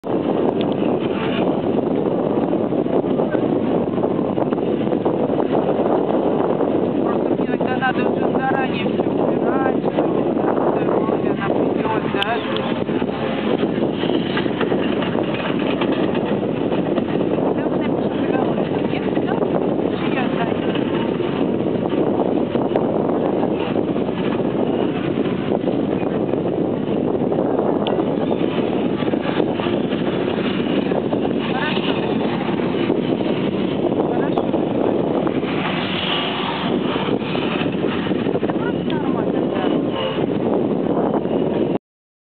View to the Finland gulf, St.Petersburg, Russia summer 2009
Let's go and see "View to the Finland gulf" St.Petersburg, Russia summer 2009. It is something new to me. No more a great triumth amaizing  impression that gives you a wish to come back in the future.